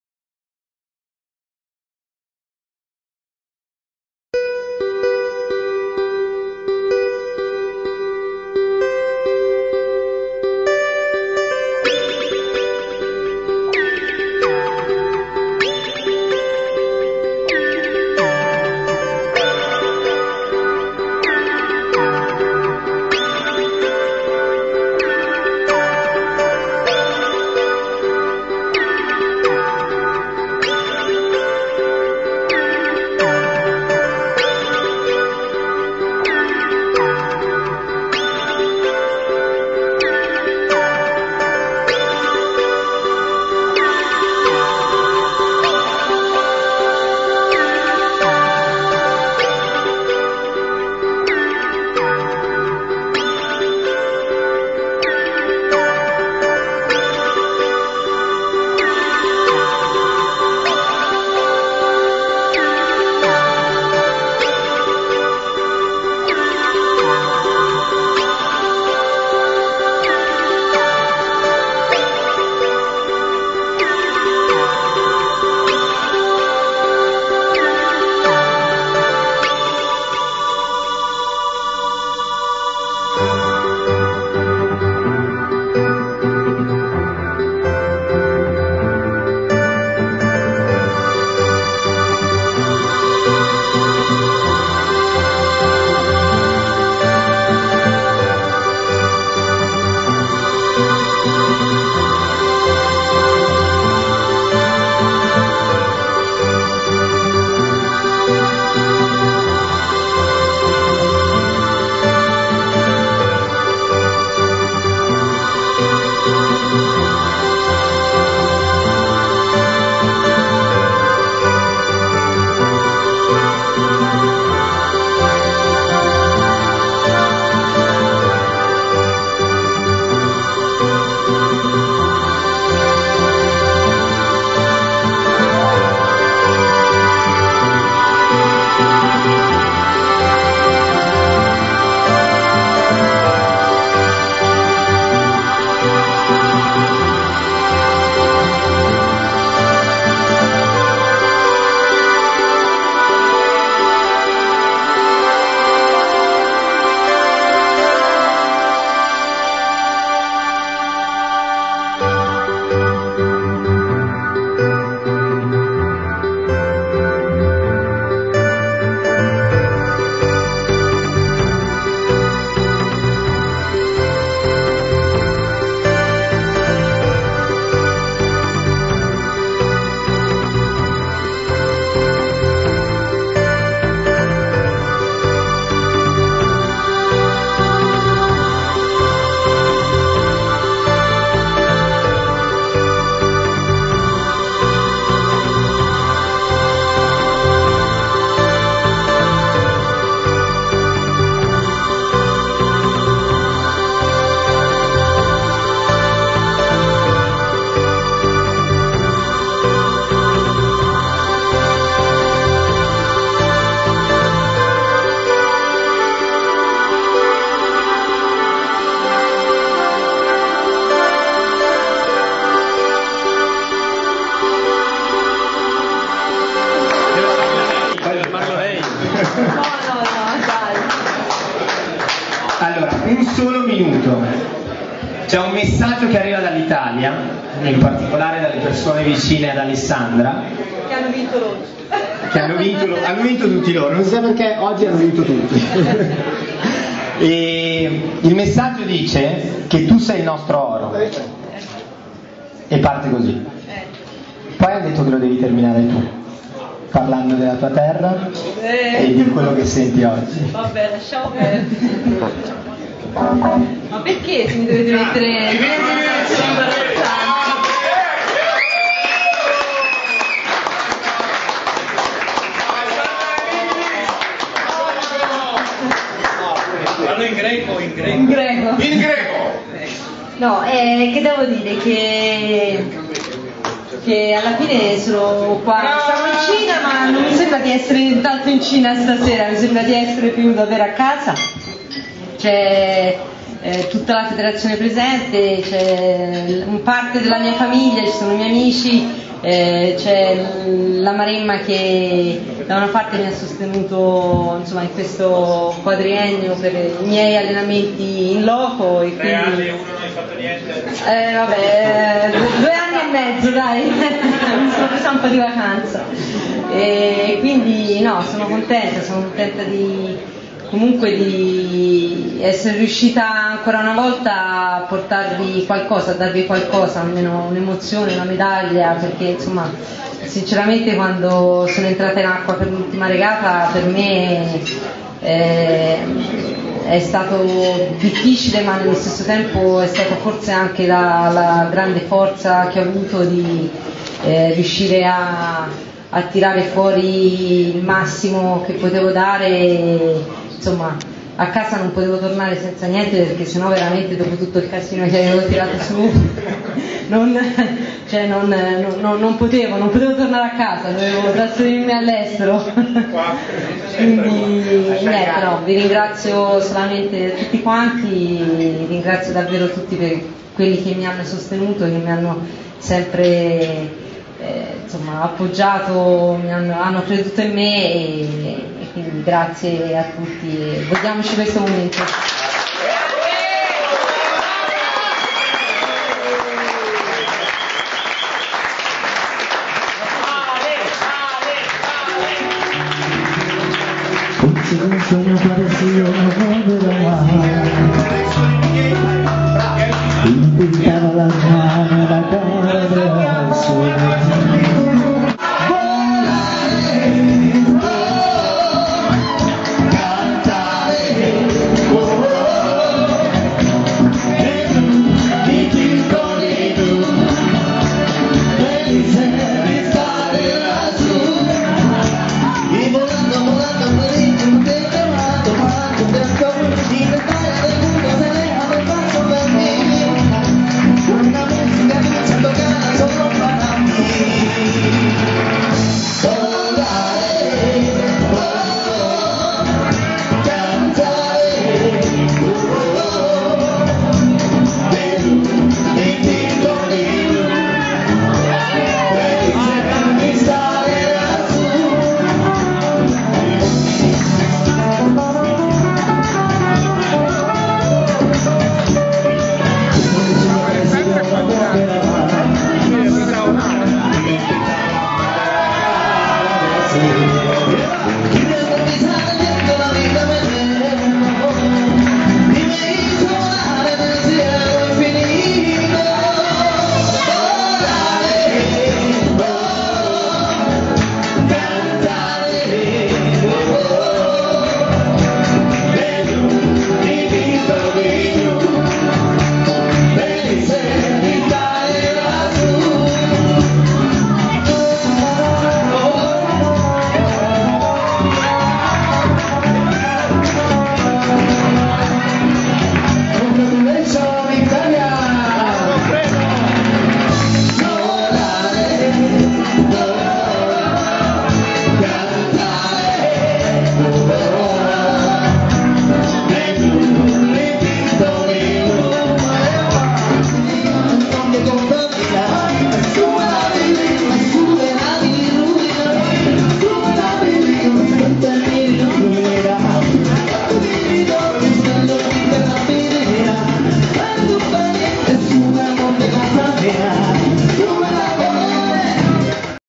Qingdao 2008: la festa italiana per Alessandra Sensini medaglia d'argento
20 agosto 2008, dopo la medaglia, la gioia e la festa.